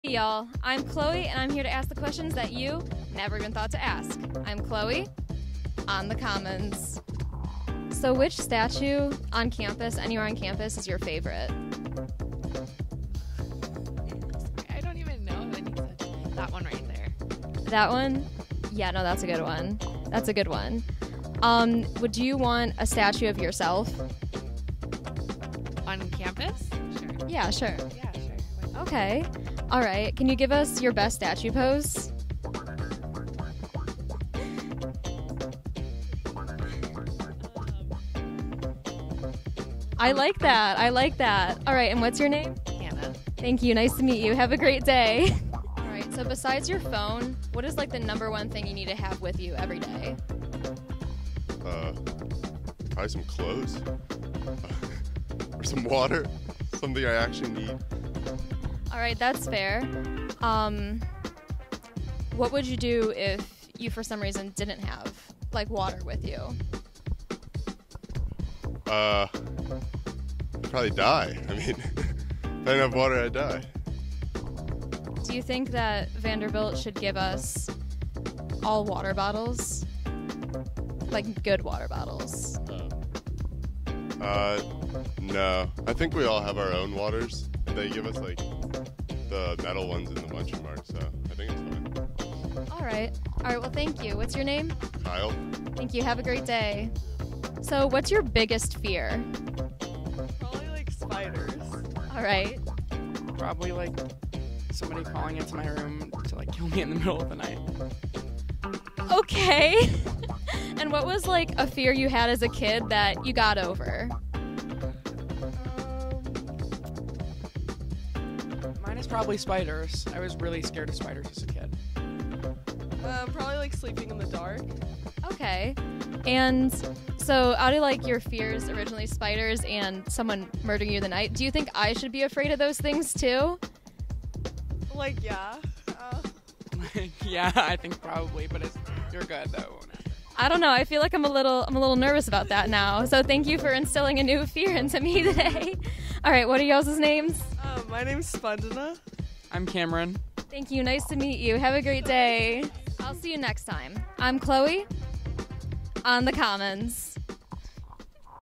Chloe on the Commons-Episode 7
Chloe took advantage of the amazing spring-like weather to head to the Commons and ask first-year students the questions you never thought to ask.  Check it out to see if your friends are featured.